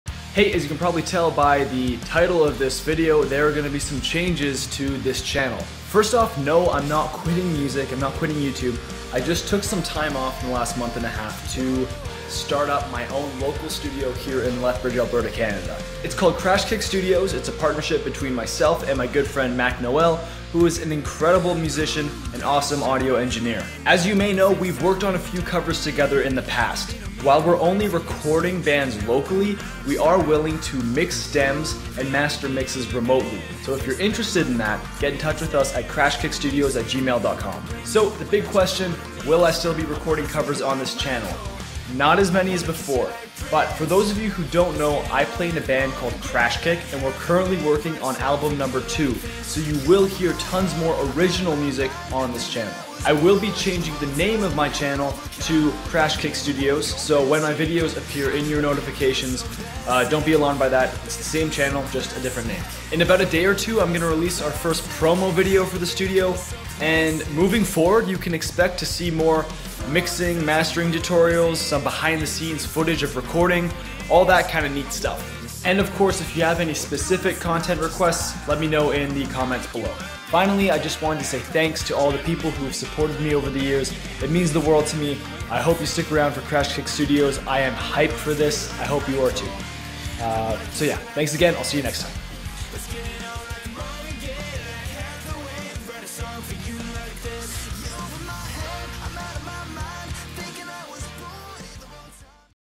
BIG NEWS!!
I will soon be changing my channel name to "Crash Kick Studios".